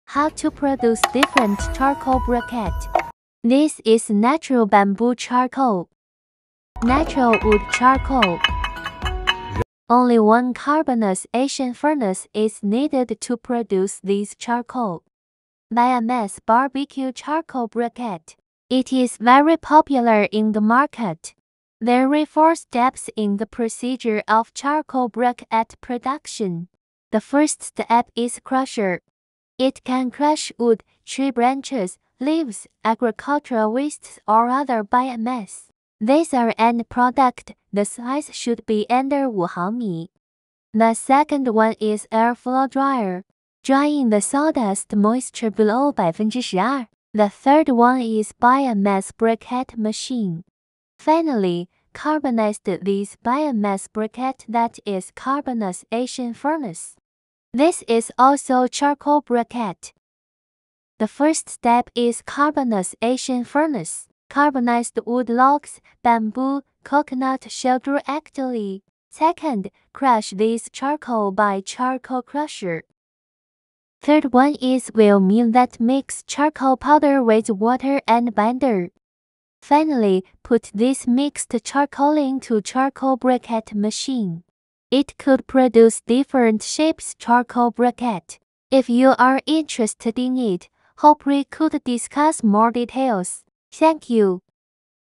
Different charcoal briquette production line
How to produce different charcoal briquette.Natural wood charcoal making machine is different from coconut shell charcoal briquette equipment.if you’re interested in making charcoal briquette production line,please contact with us.
Hope we could discuss more details
 Sarah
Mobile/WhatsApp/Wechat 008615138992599